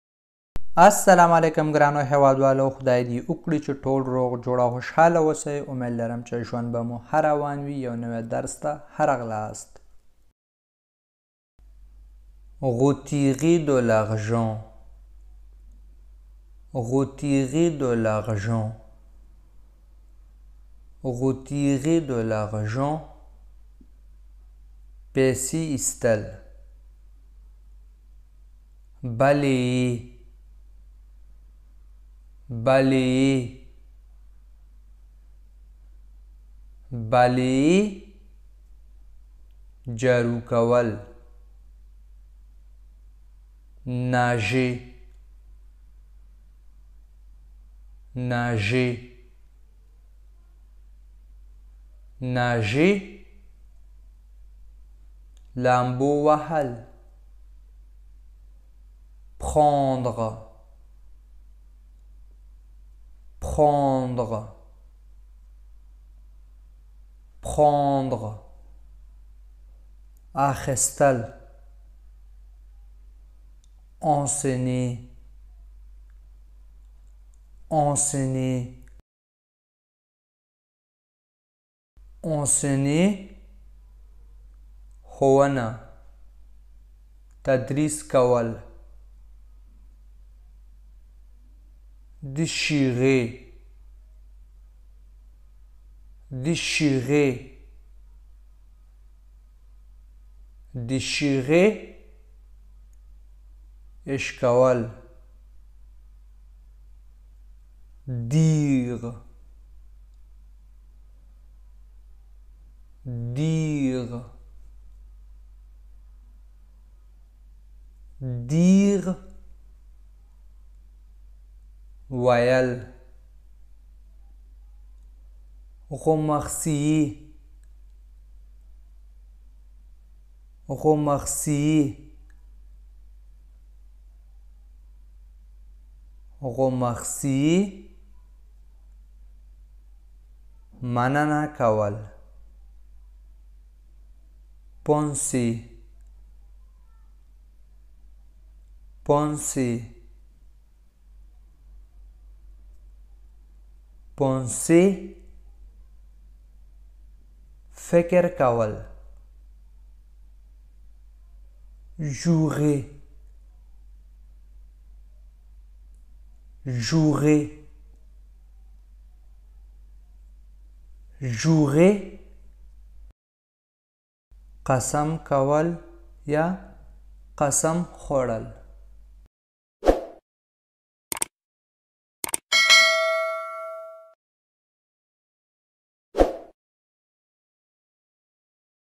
Leçon 365 : Learn French in Pashto - فرانسوي زده کړه په پښتو ژبه کې - د فرانسوي ژبي لغاتونه
Learn French in Pashto - فرانسوي زده کړه په پښتو ژبه کې - د فرانسوي ژبي لغاتونه

I am Fazal Kareem welcome to my chenal.

 About this video :
Les 14 Verbes les plus utilisés en Français.

فرانسوي ژبې لغاتونه او جملي